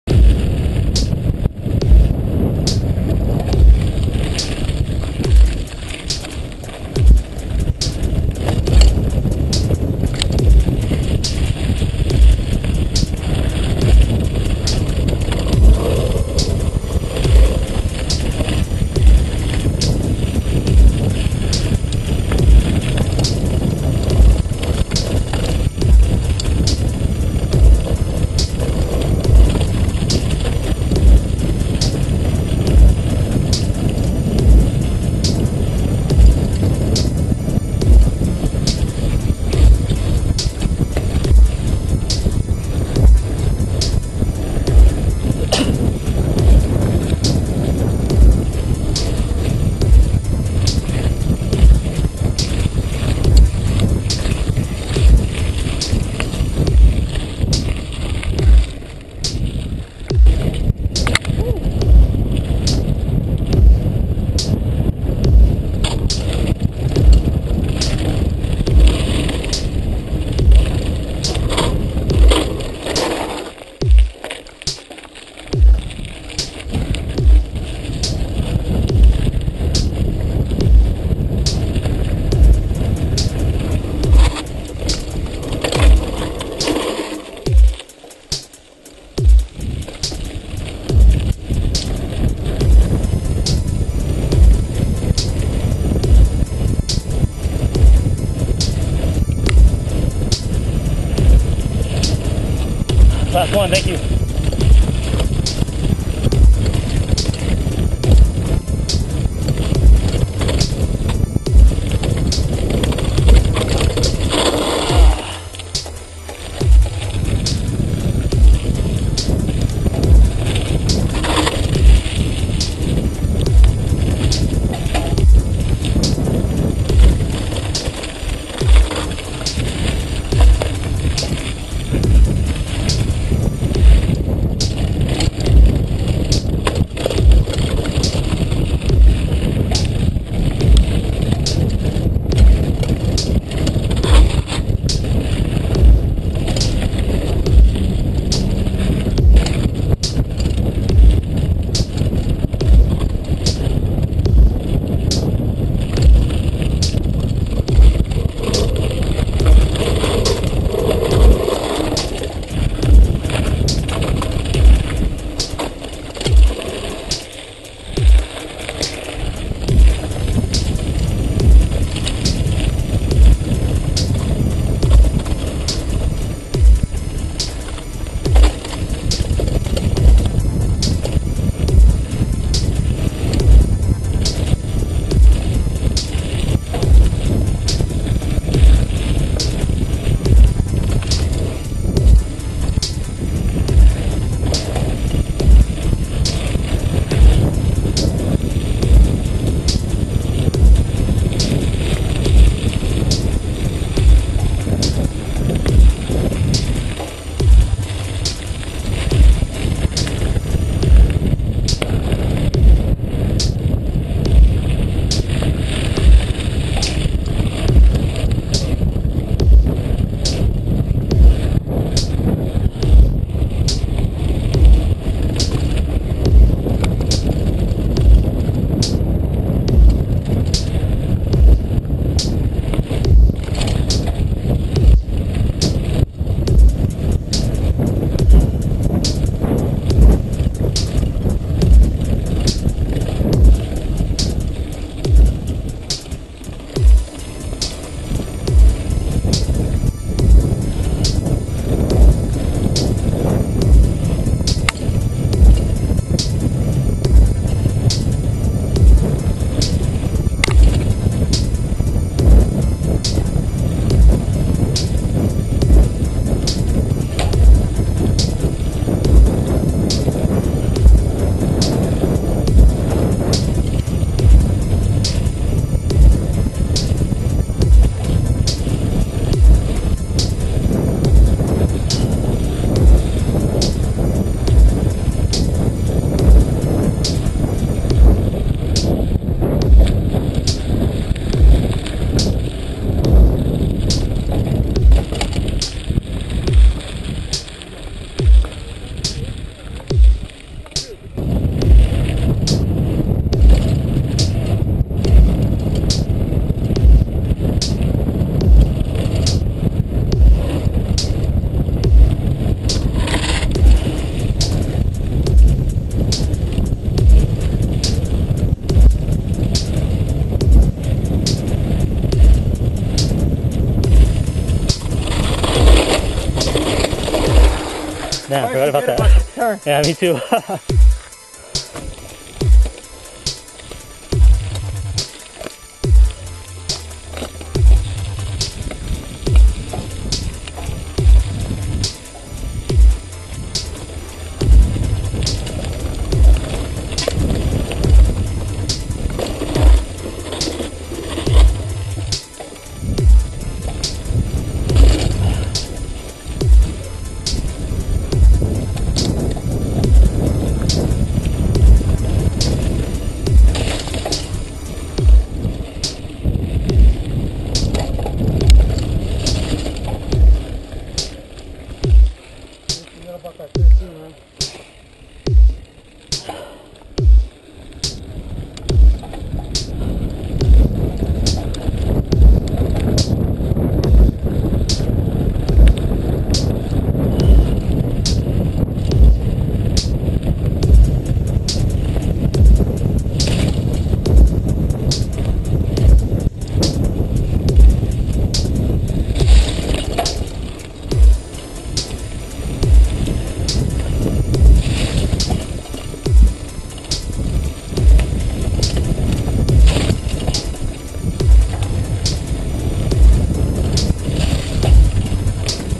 Giant Anthem X 29er POV at Marshall Canyon Mountain Biking - 2013 HD
Getting back on the bike after 3 months off.  Pushing the limits to get back into shape for the 2013 race season :)  Riding my Giant Anthem X 29er